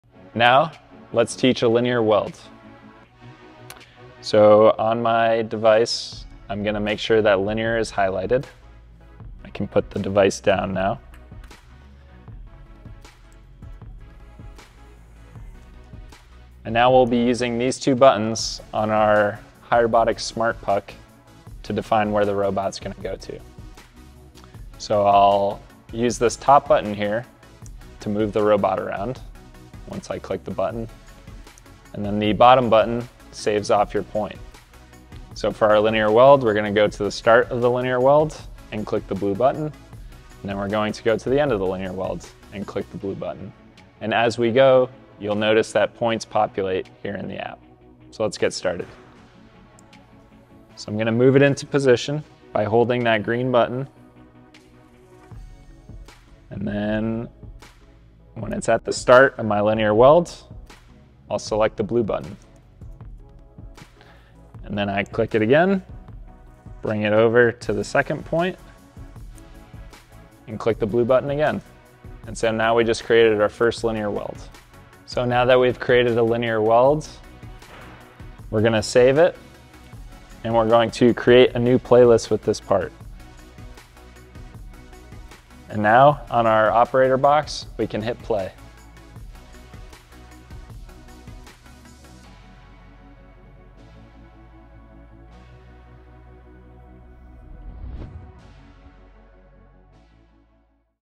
Program Linear Welds in 90 Seconds - Lesson 2 | Cobot Welder
Optimized Title: "Program a Linear Weld in 90 Seconds - Lesson 2: Teach Your Robot to Weld Straight Lines | Cobot Welder"

Optimized Description:

Watch how easy it is to teach a collaborative robot to weld perfectly straight lines using just two buttons - no programming code required! This step-by-step tutorial shows you the exact process to create linear welds that are faster and more consistent than manual welding.

What You'll Learn:
 – Program linear welds using the intuitive Smart Puck interface
 – Move and position your cobot welder with simple button controls
 – Create precise, repeatable weld paths in under 2 minutes
 – Save and organize weld programs for future production runs

Perfect For:
 ✅ Welders learning collaborative robot programming
✅ Fabrication shops automating repetitive linear welds
✅ Small manufacturers wanting consistent weld quality
✅ Anyone amazed that robot programming can be this simple

Why Linear Welds Matter:
 – Linear welds are the foundation of most fabrication work - from structural steel to sheet metal assemblies.
 – Automating these welds eliminates fatigue, reduces rework, and lets your welders focus on complex manual tasks while the cobot handles repetitive production.

0:00 - Setting up linear weld mode in Beacon app
0:15 - Using the Smart Puck control buttons
0:24 - Programming weld start and end points
0:50 - Positioning the robot for first weld point
1:06 - Setting the second point to complete the weld path
1:16 - Saving your linear weld program
1:22 - Creating playlists for production efficiency
1:28 - Running your automated linear weld

Ready to see if cobot welding fits your parts?

Next Up: Learn radial welds, tack welds, and advanced programming techniques in our Complete Cobot Welding Course playlist!